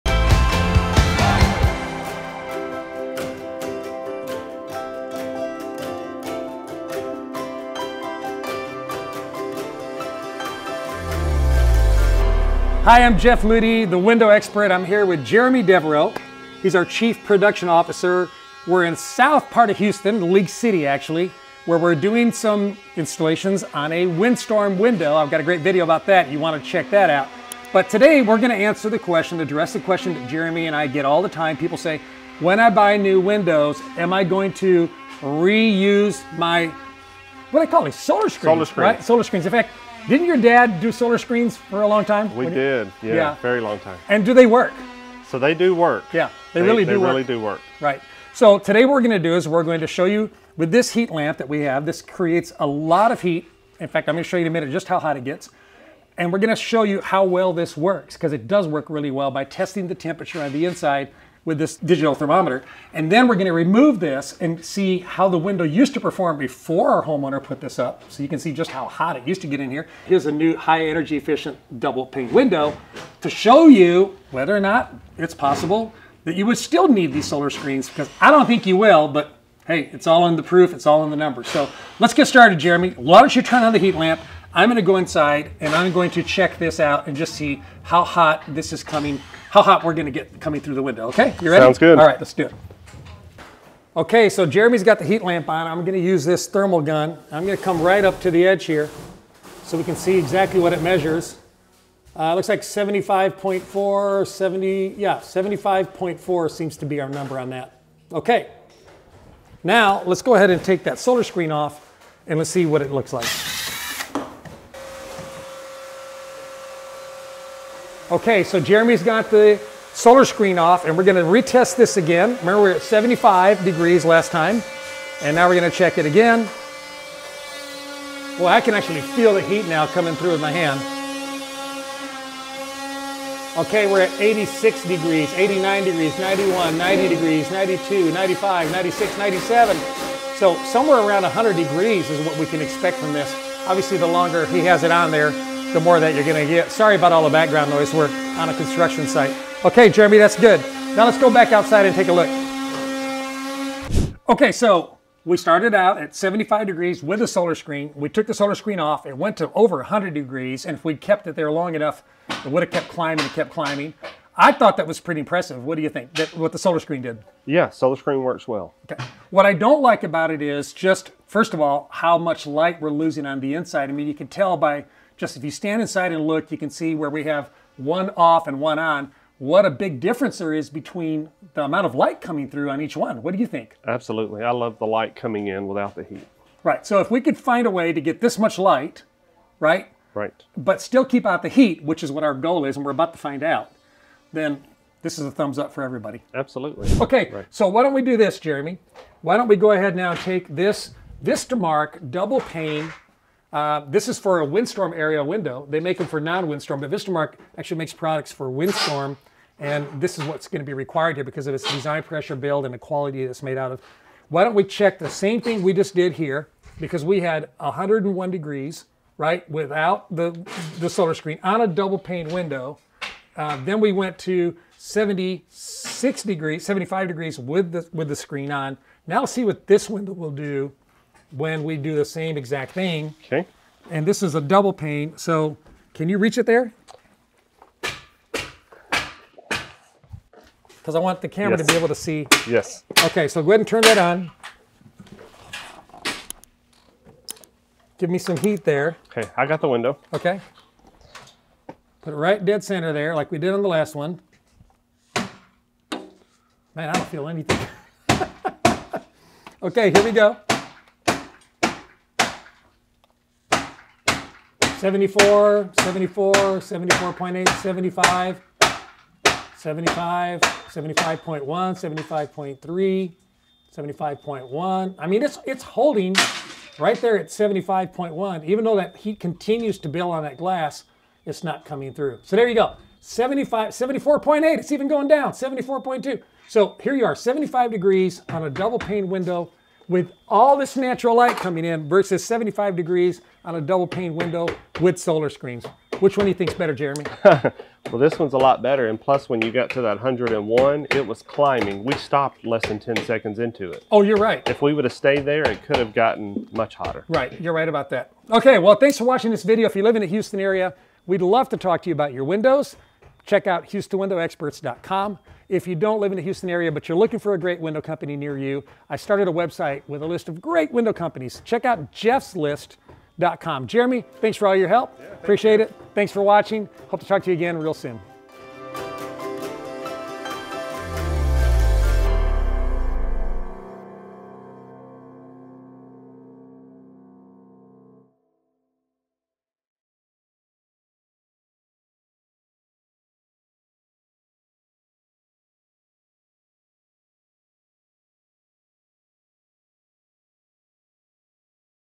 Solar Screen VS Double Pane | Which One Is Better?
We check the heat efficiency of the Vistamark double pane window so you don't have to. We compare it to a basic builder grade double pane window with and without solar screens. This video will definitely help you make an informed decision if heat transference is an issue you are trying to solve.

CHAPTERS
00:00 - Intro
01:22 - Solar screen heat test
01:57 - Basic window heat test
03:25 - Visamark window heat test
05:07 - outro